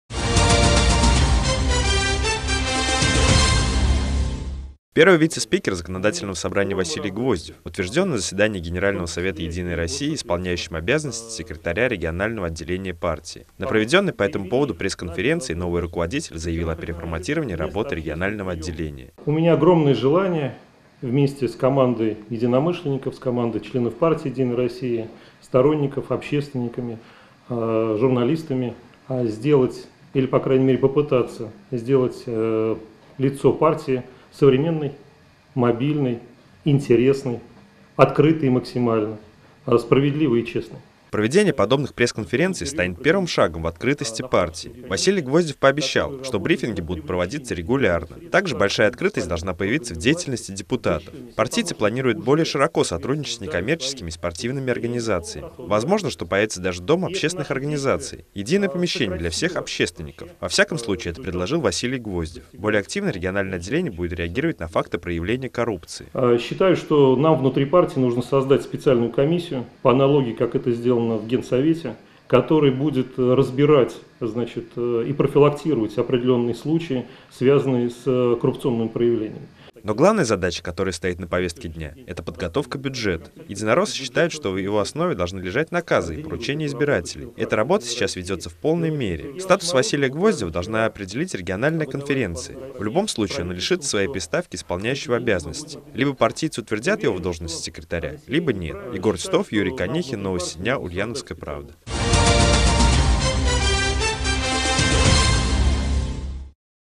Василий Гвоздев: «Мы хотим сделать лицо партии открытым и современным»
Первый вице-спикер Законодательного Собрания Василий Гвоздев утверждён на заседании Генерального совета «Единой России» исполняющим обязанности секретаря регионального отделения партии.